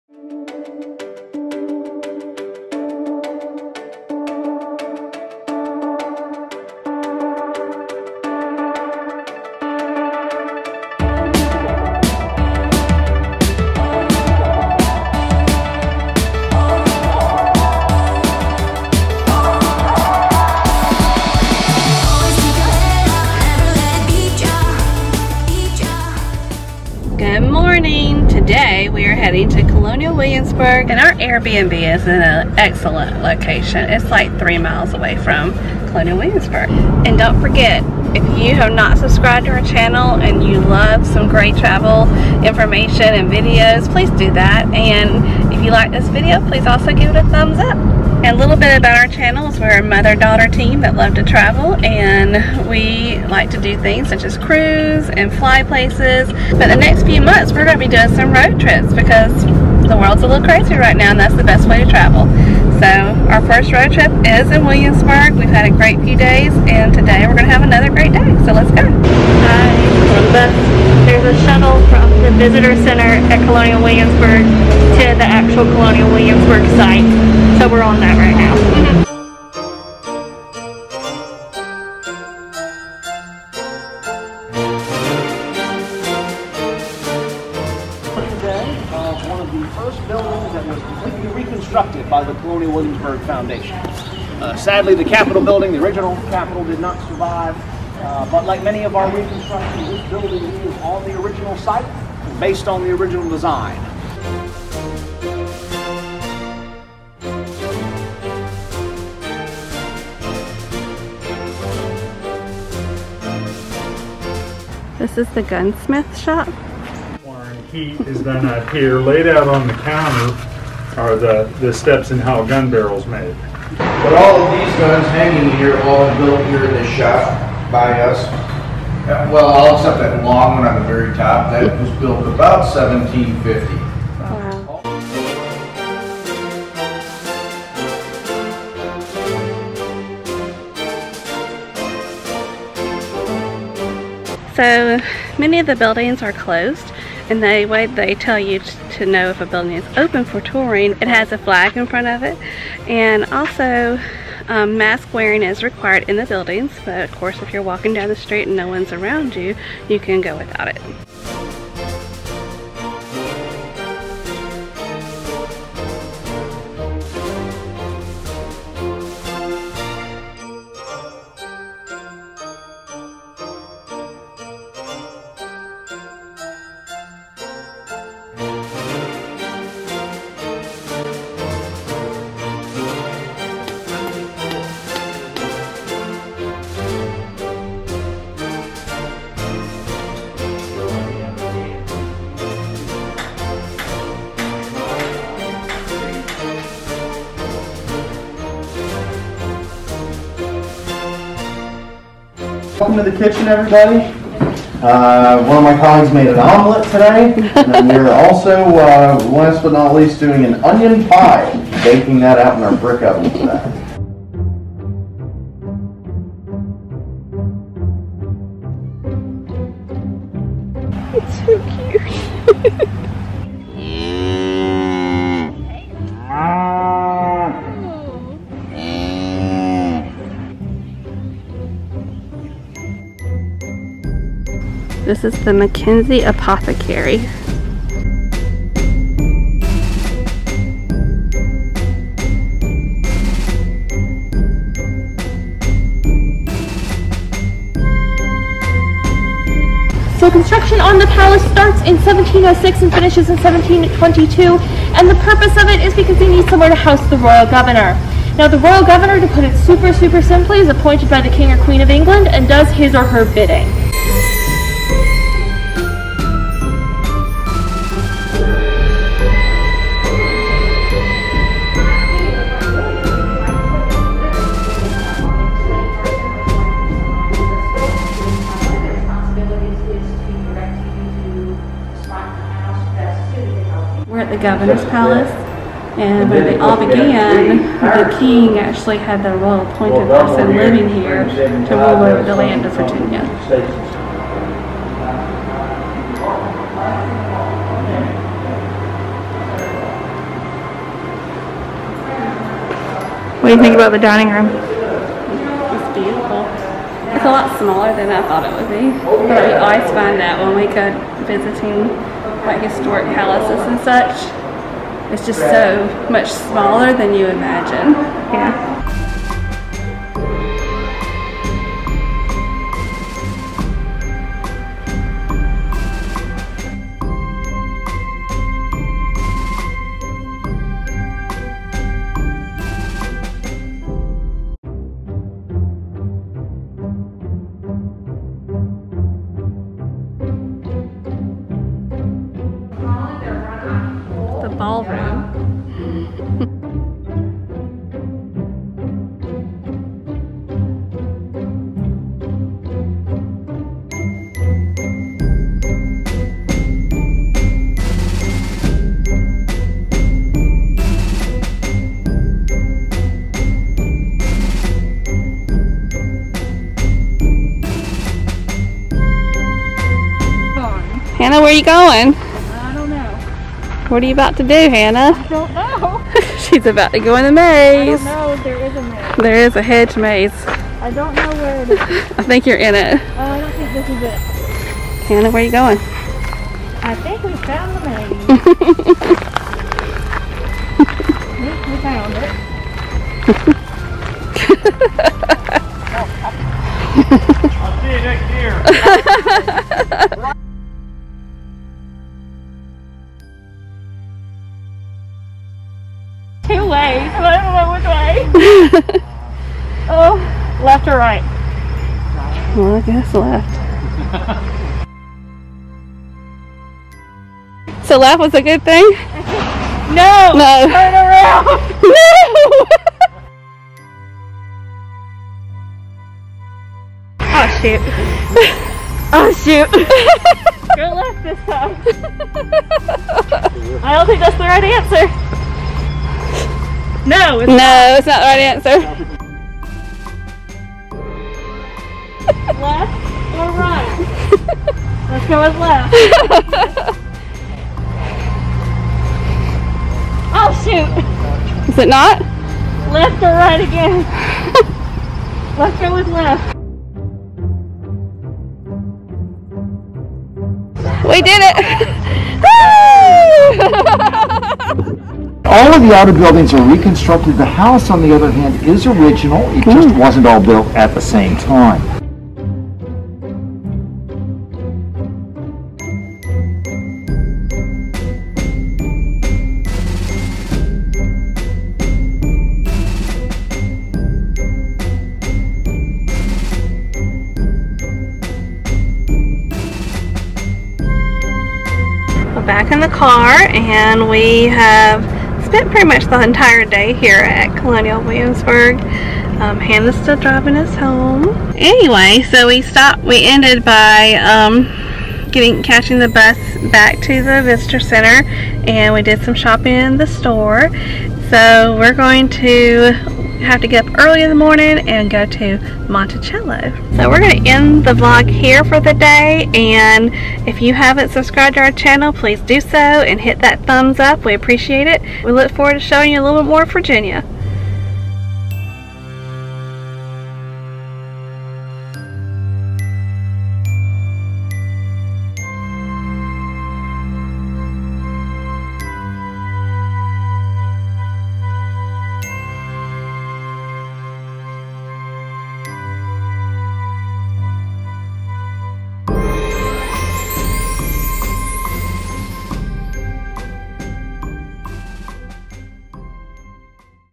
Williamsburg, Virginia- Let’s Explore Colonial Williamsburg
On the fourth day of our trip to Williamsburg, Virginia, we explored Colonial Williamsburg and enjoyed viewing the reenactments of life in the settlement and experiencing the way of life in Colonial times. In this series, we will take you on the historic triangle of the Jamestown Settlement, Colonial Williamsburg and Yorktown.
Let's explore our trip to Williamsburg, VA!

We LOVE getting mail!
Laurie and Hannah
1450 Georgia Avenue